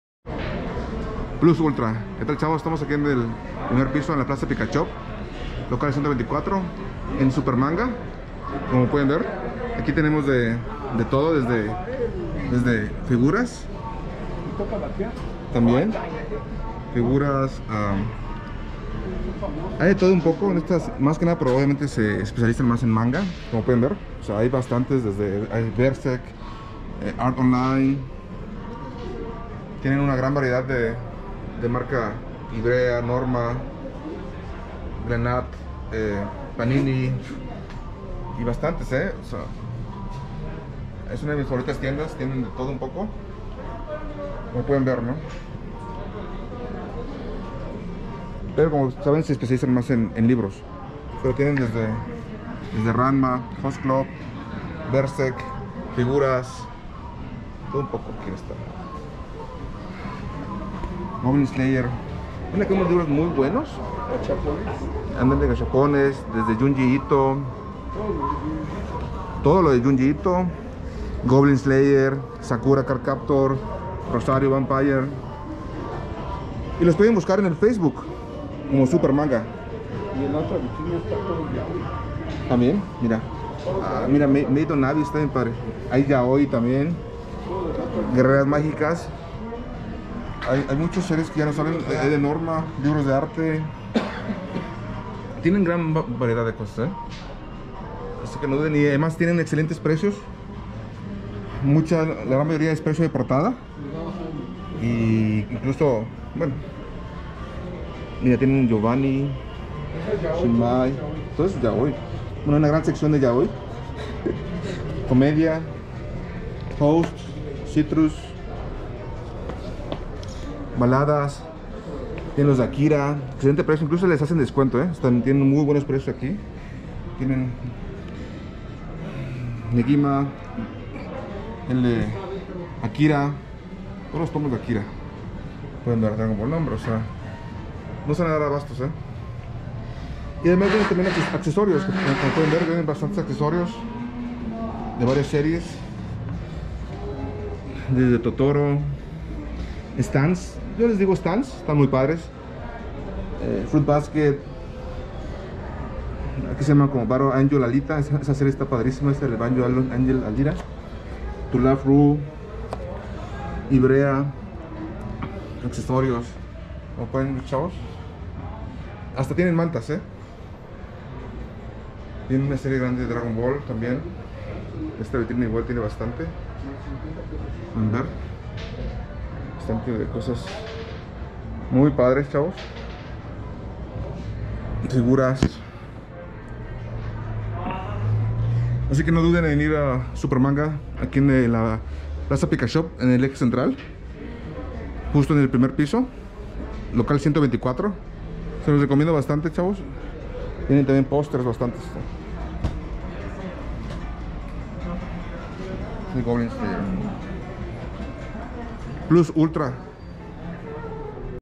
Supermanga, plaza pikashop CDMX 2021 part 1
Supermanga, es una tienda de manga, accesorios , novelas graficas y mas. Ubicada en el eje central Lazaro Cardenas # 12 , dentro de la plaza pikashop local 124.